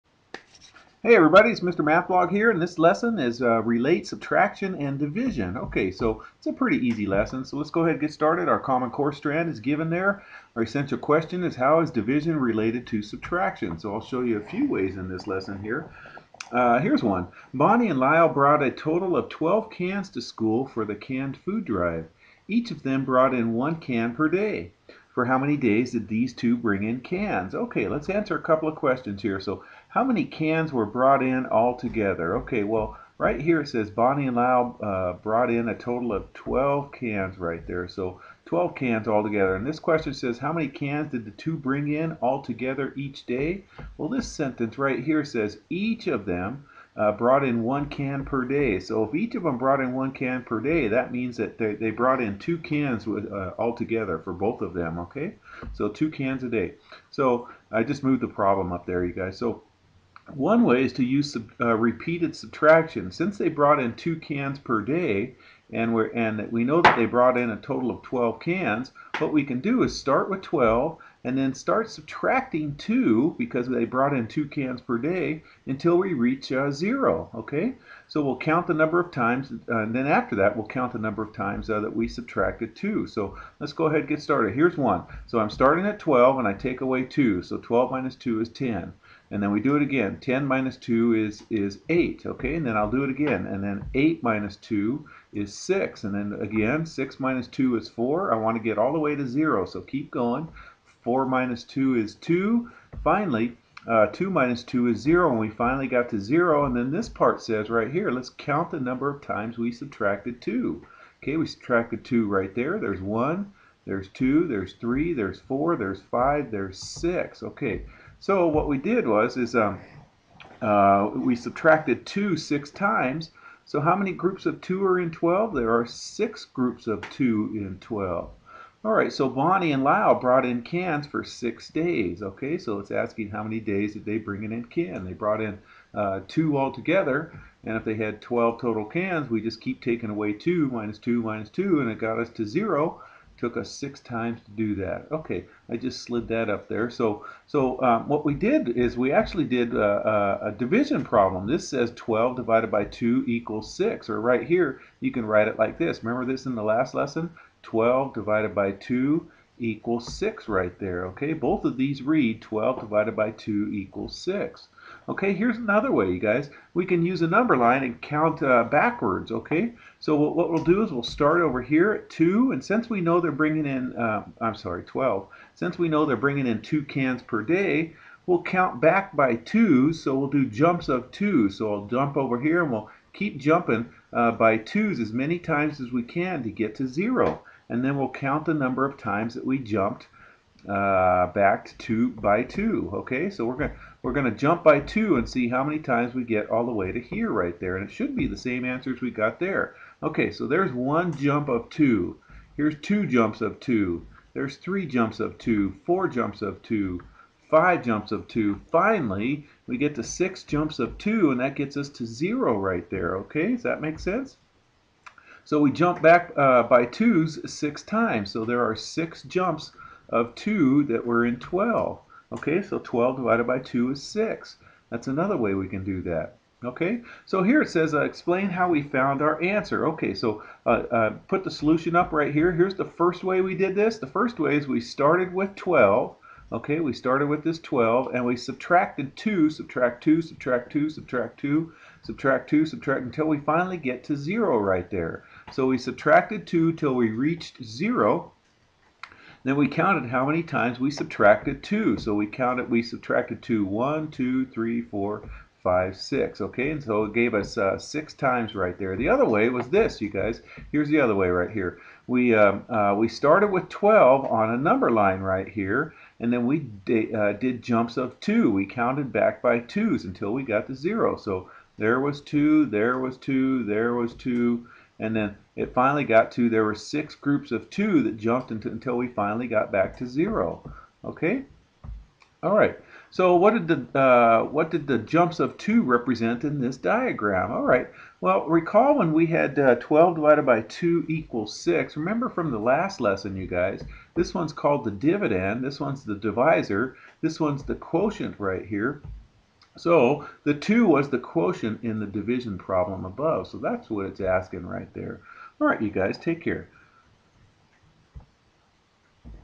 Relate Subtraction and Division - Lesson 6.5
This lesson shows you how you can subtract numbers to show division.